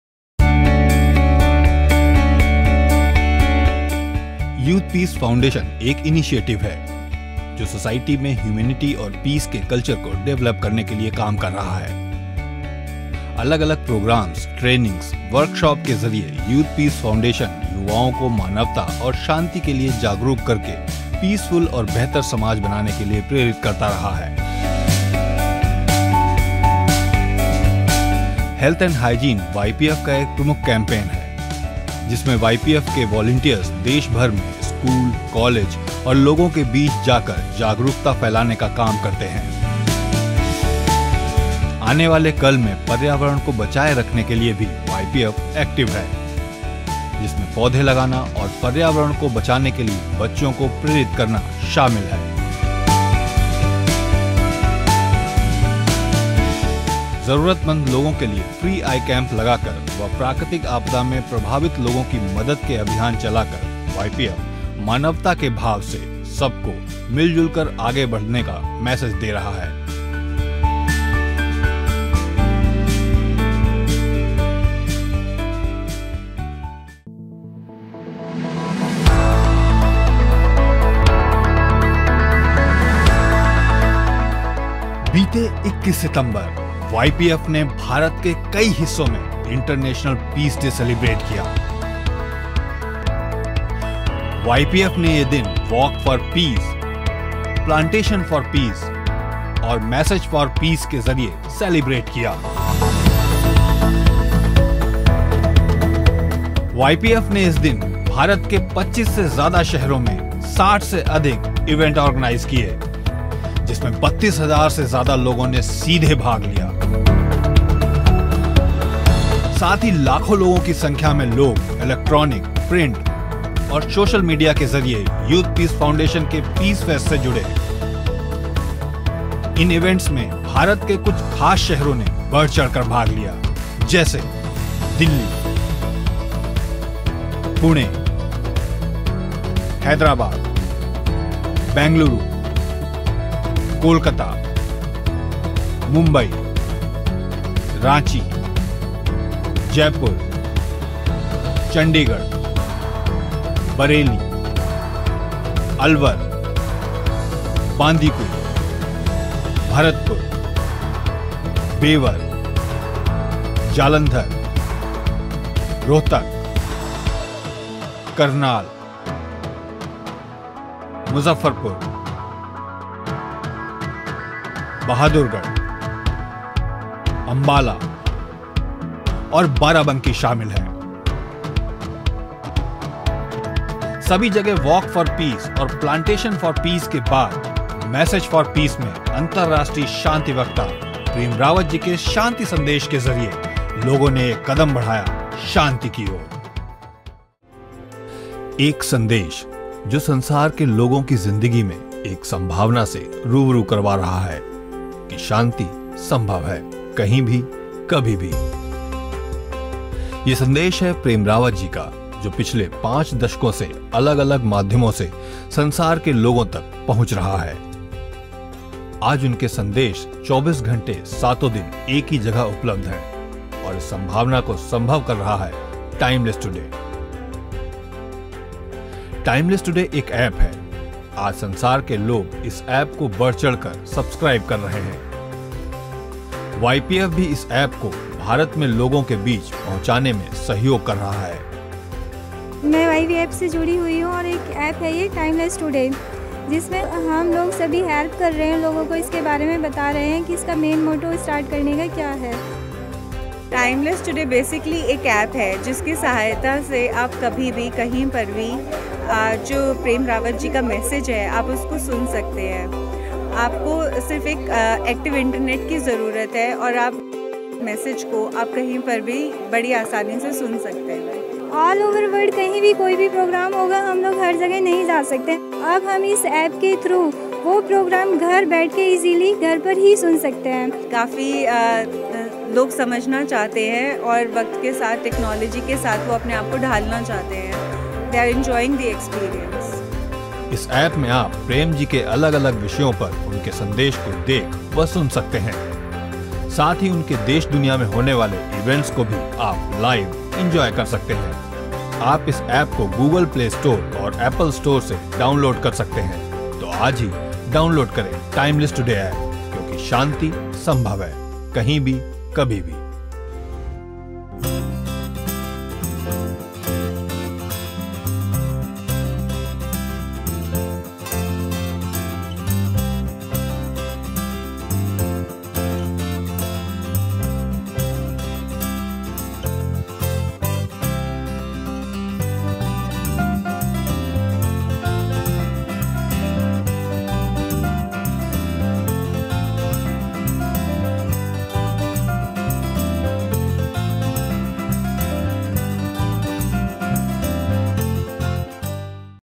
Youth Peace Foundation | Short Intro
Youth Peace Foundation (YPF) campaign is a commitment towards building a culture of humanity and peace. In the rapidly changing life where studying, work, and technology are constantly challenging them, young people have little time to consider their own peace and well-being.

We are invoking thoughts and developing the culture of peace amongst youth through various activities, training, and workshops. Trying to help individuals achieve peace in their lives.